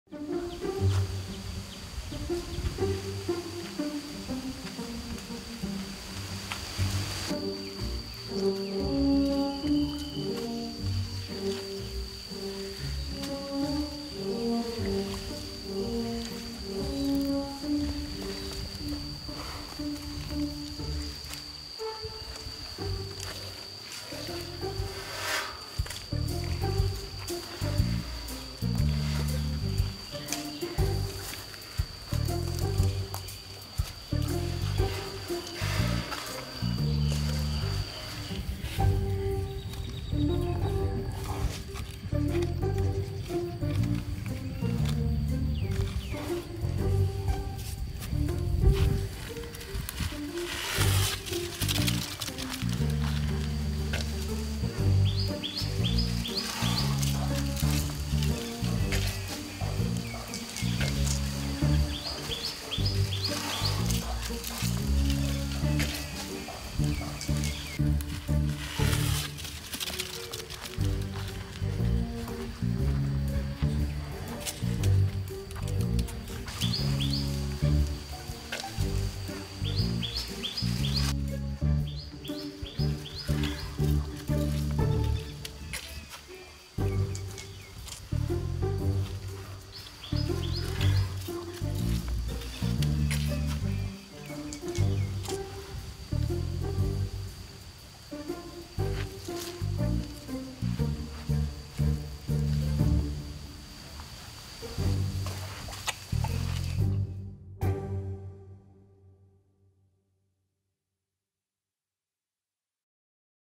Baby Elephant Pyi Mai Learn How To Get And Her Fruit From Fig Tree - ElephantNews
A fig tree offers the perfect scratch post for a baby elephant such as PyiMai. And surprise ! surprise ! There are treats to be found hidden like delicious little treasures among the twisted roots, and entwined tree branches which, with a good exploratory stretch, she can just reach to whet her appetite for the sweet fruits at Elephant Nature Park.